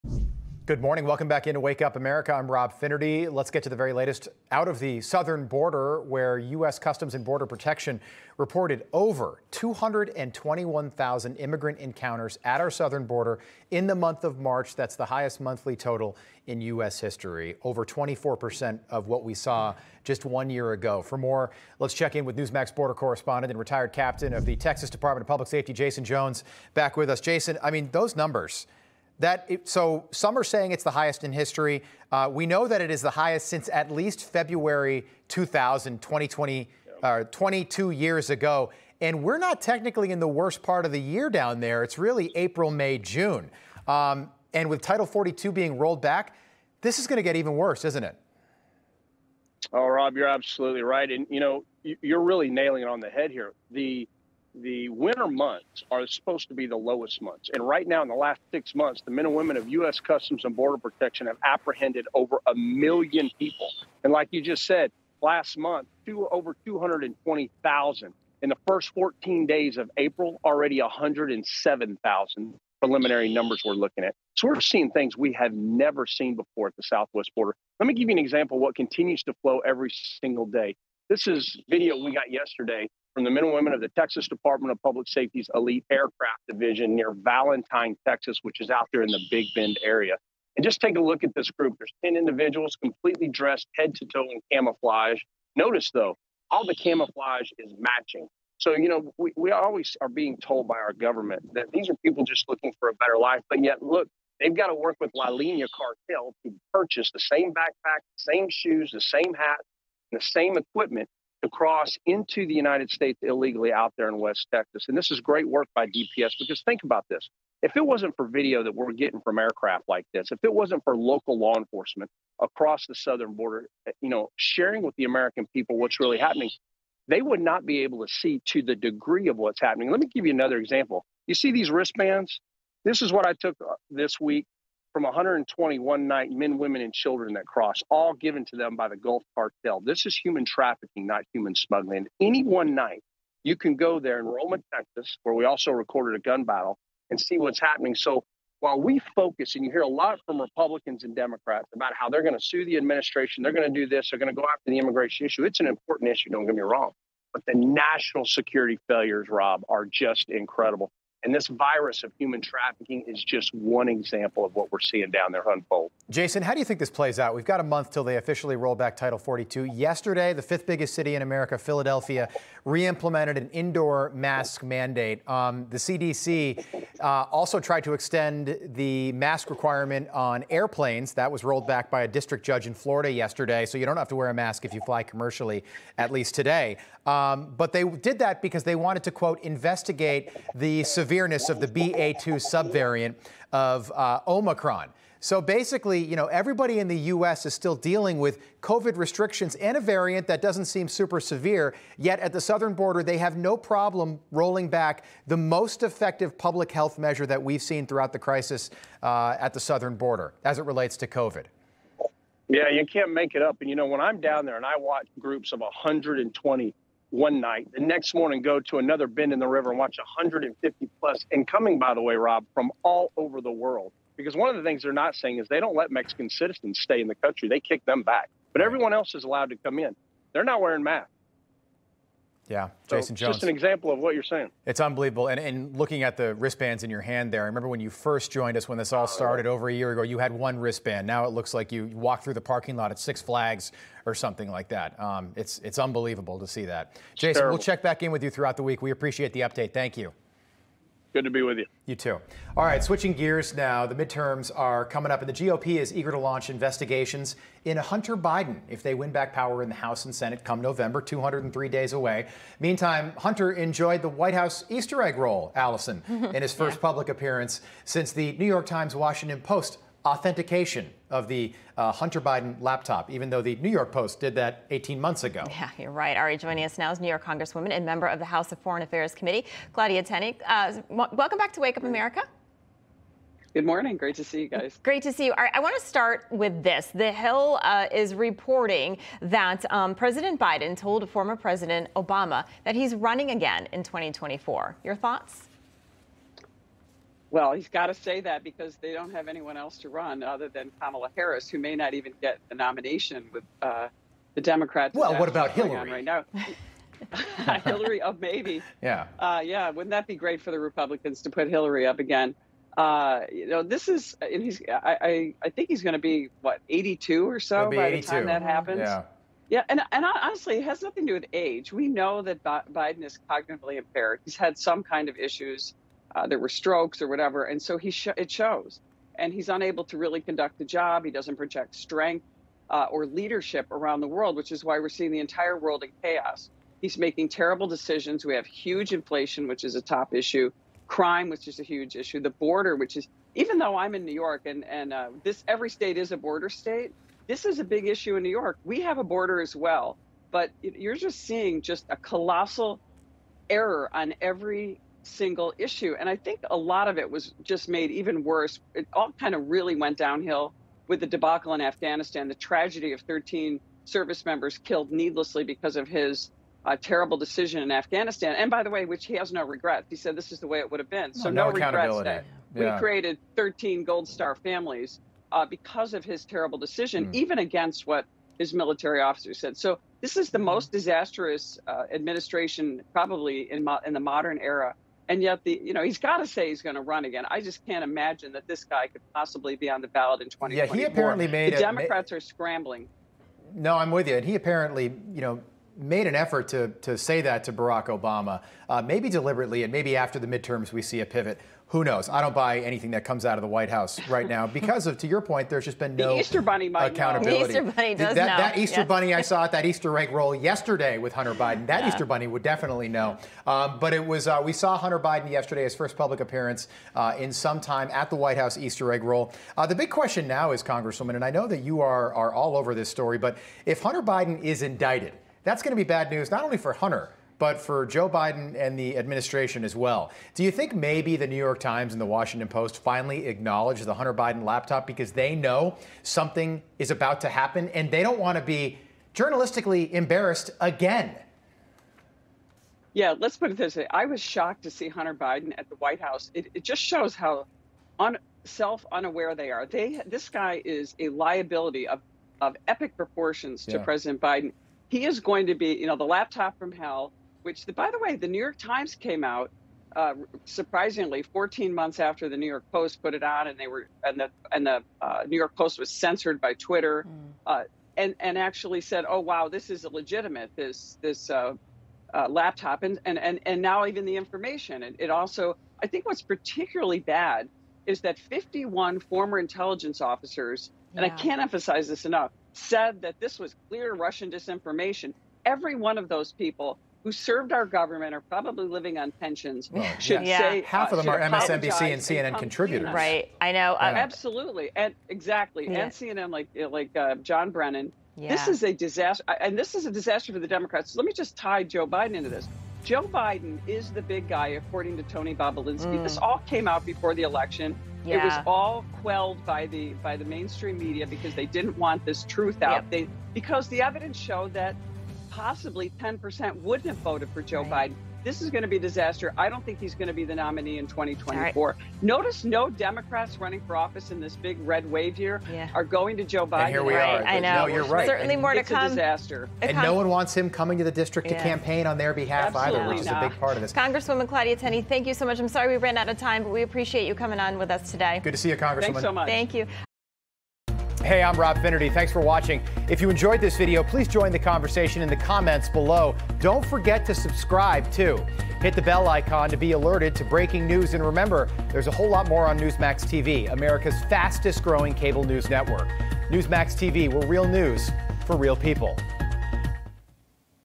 Biden tells Obama he's running again
Rep. Claudia Tenney reacts to President Biden telling fmr. President Obama that he's running again in 2024.